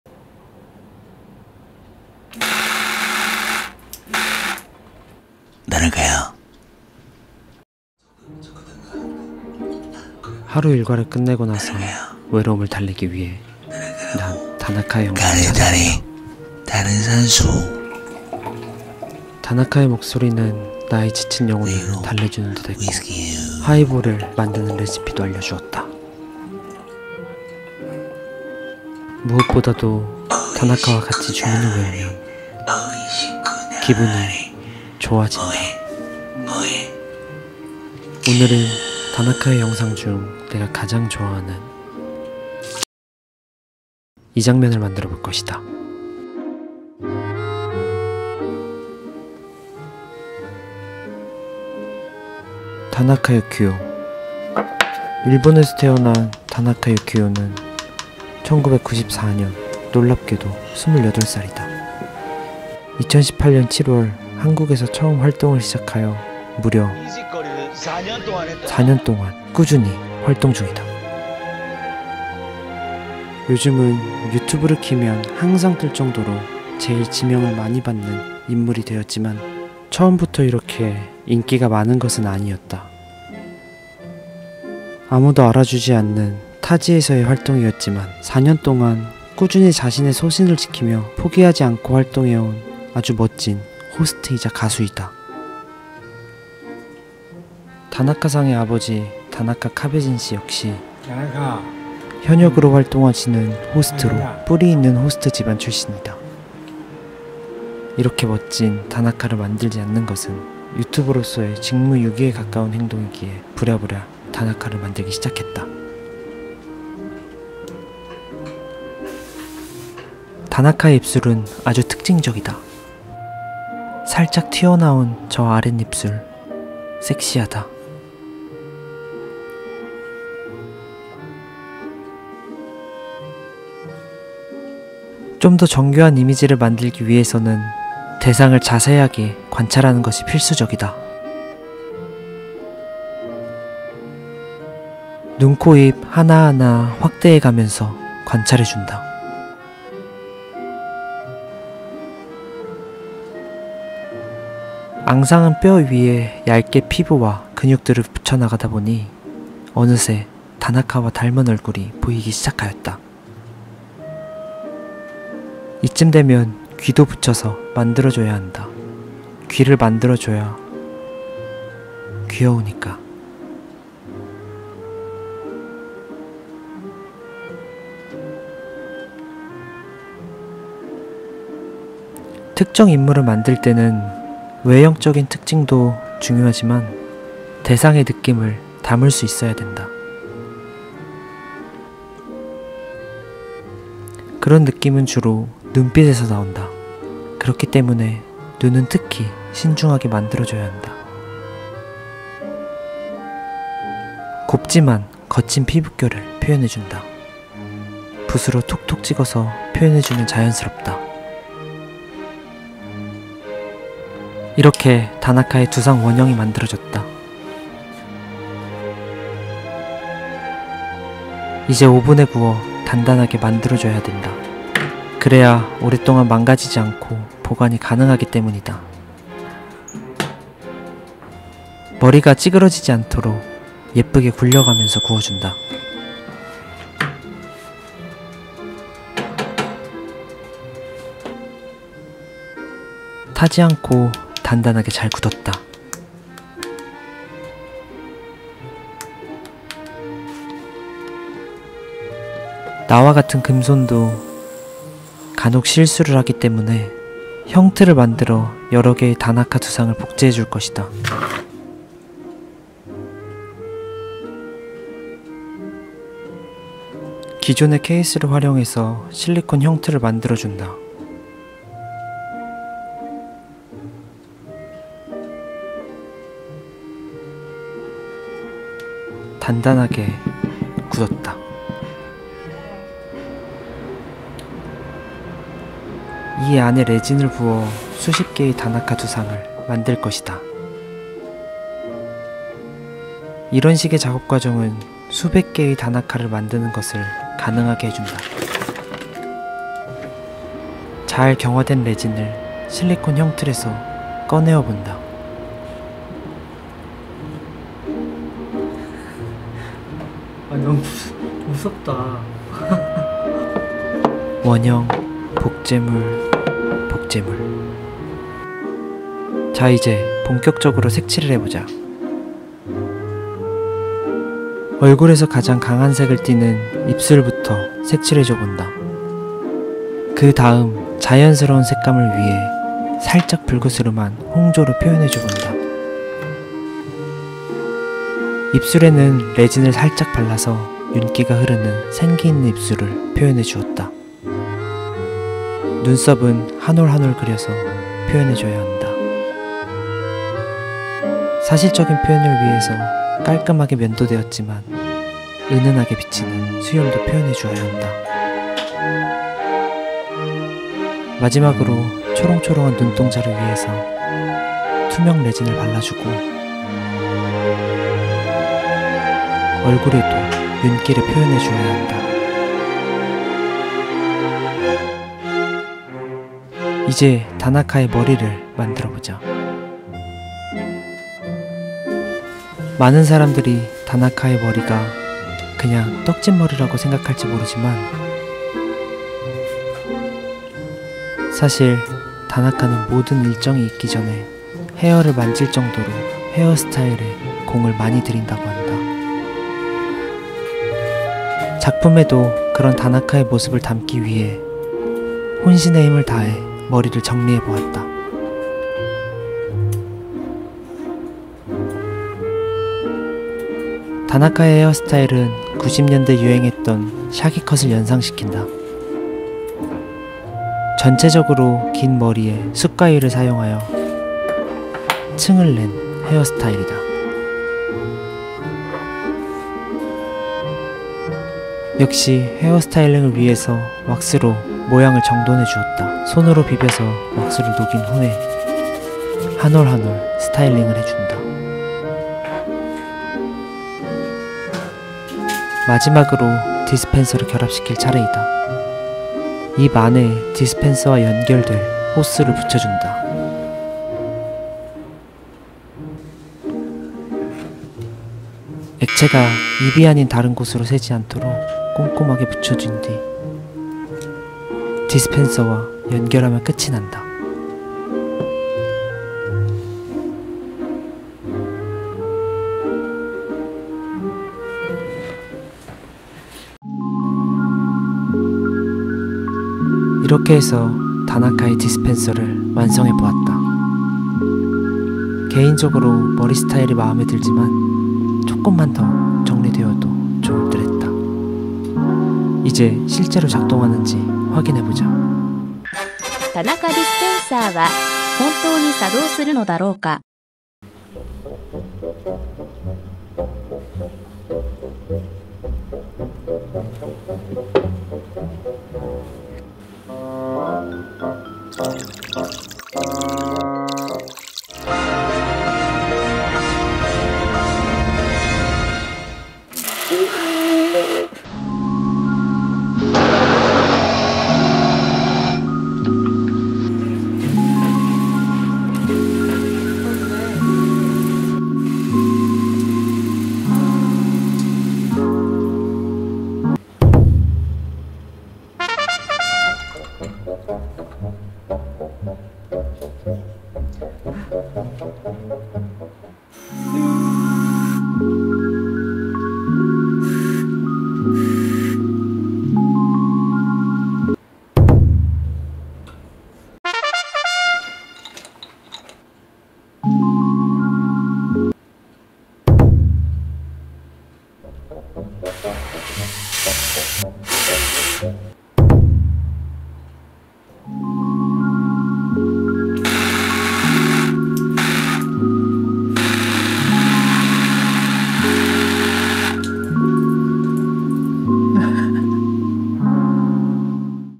꼬ㅊ 미남 '다나카 디스펜서' 만들기
* 영상 사용을 허락해주신 다나카님께 감사를 전합니다.*

00:00 Intro
00:46 Clay modeling
04:09 Mold making
04:43 Resin casting
05:18 Coloring
06:14 Making Hair
07:23 Dispenser
07:56 Final
08:15 Test dispenser